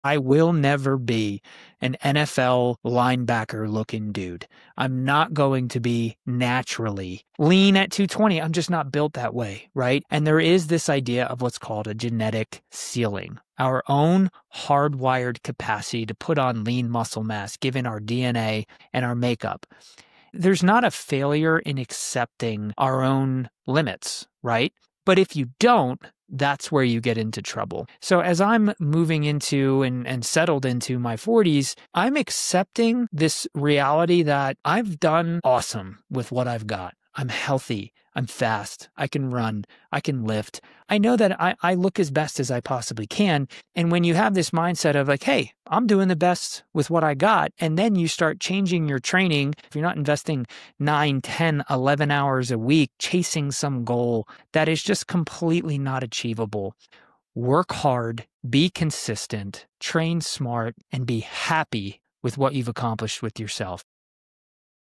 What is a Genetic Fitness Ceiling? (And Why You Should Embrace It)
A majority of the professional athletes we aspire to emulate are hard working humans who happened to also hit the genetic lottery. Most of the fitness influencers we find online use pretty aggressive “supplements” to achieve their results.

There’s a limit to how much lean muscle your body can NATURALLY build — it’s called your genetic ceiling.

Science shows most men can gain around 20–40 pounds of lean muscle in their lifetime (women, roughly half that), depending on genetics, training quality, and recovery. Beyond that point, progress slows — not because you’re lazy or doing it wrong, but because your body’s blueprint sets boundaries for growth.

That should be liberating for you - not discouraging

Your goal should never be to look like someone else. It’s to push your own personal potential — to train, eat, and recover like someone who respects their biology, as best YOU can.

Don’t waste years chasing someone else’s ceiling.
Build under your own, and be proud of every inch of strength you earn.